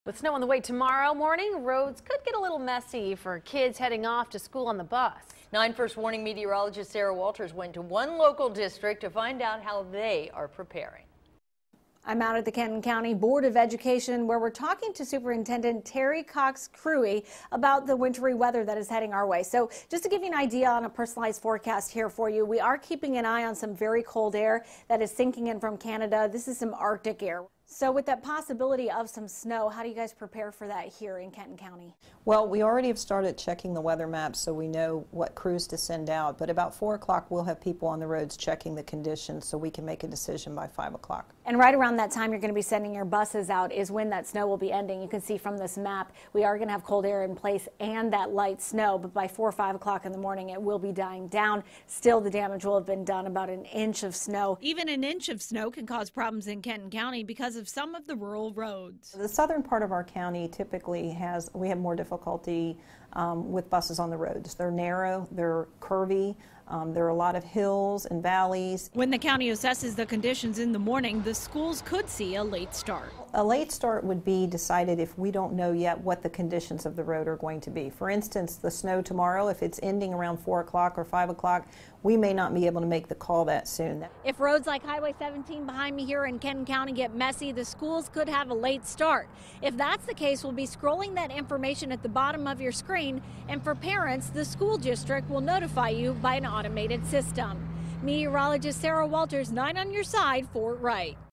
Snow day or delay? How schools decide
WCPO meteorologist Sarah Walters talks to a superintendent.